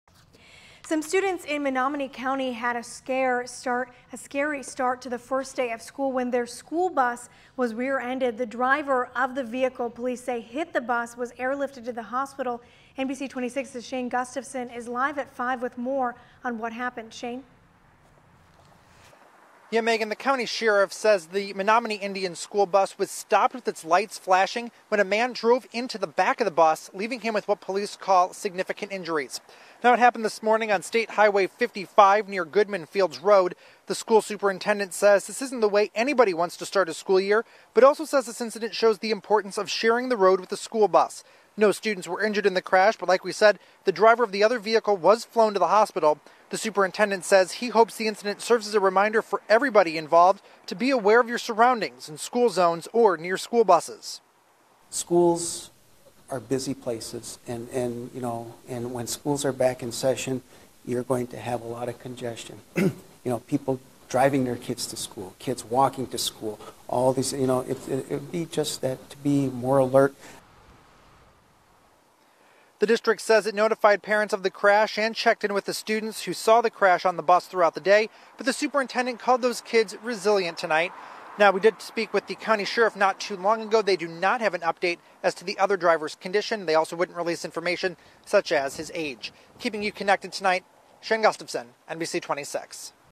Driver injured after rear-ending school bus in Menominee County
A driver suffered significant injuries after rear-ending a school bus in Menominee County Tuesday morning. Around 7:30 a.m., a Menominee Indian School District school bus was stopped with its lights flashing on STH 55, the Menominee County Sheriff's Department said. The bus was rear-ended by one vehicle, deputies said.